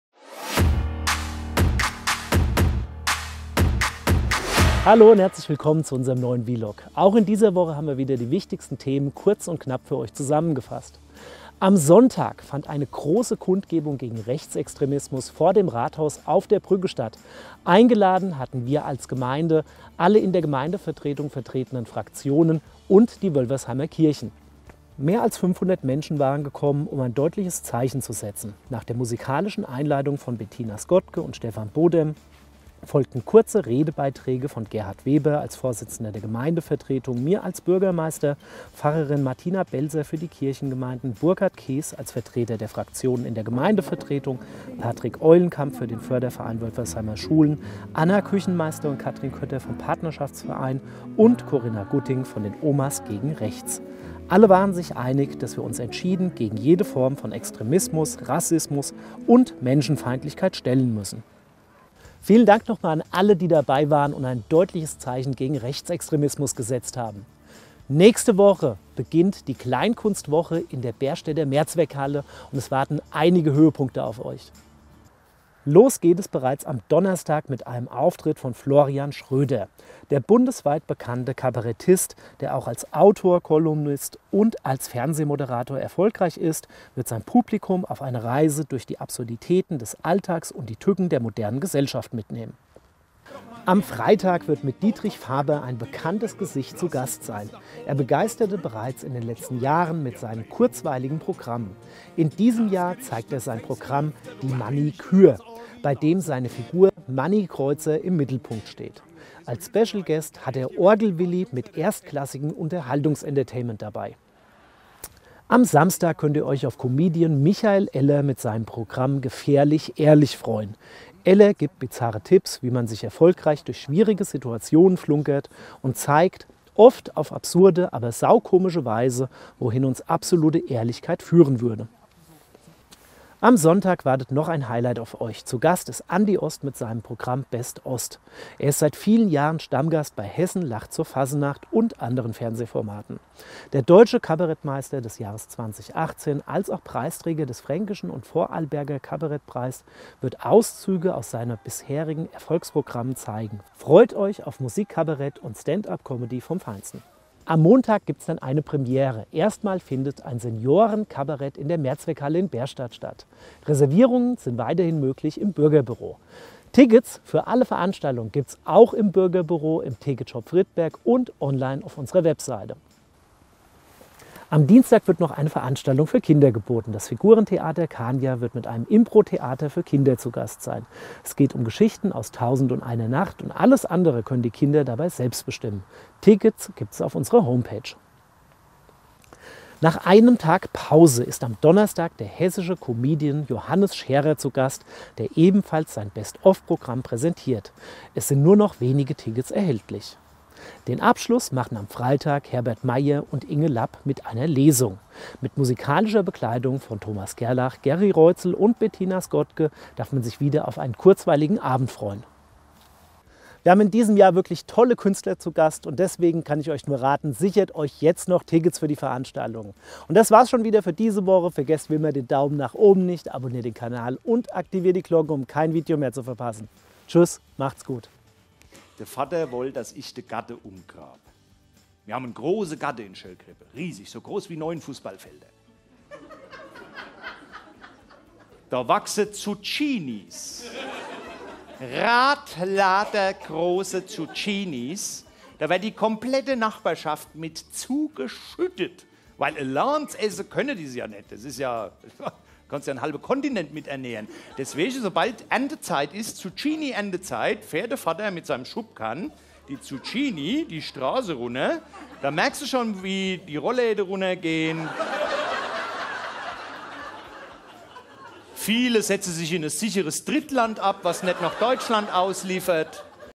Wölfersheim - VLOG #194
In dieser Woche gehen wir auf die zurückliegende Kundgebung vor dem Rathaus ein und berichten ausführlich über die bevorstehende Kleinkunstwoche.